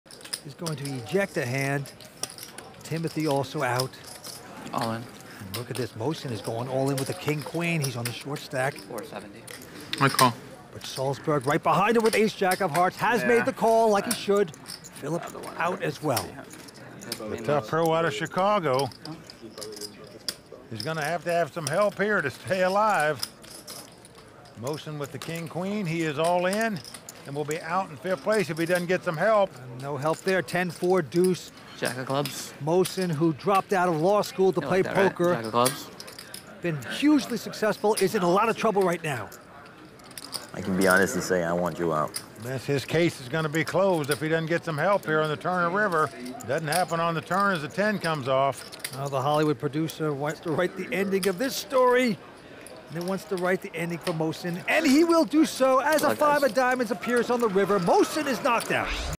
Mohsin Goes ALL IN With a King Queen, Hoping to Get Lucky! 🫣
If the river card doesn't save him, he will be eliminated!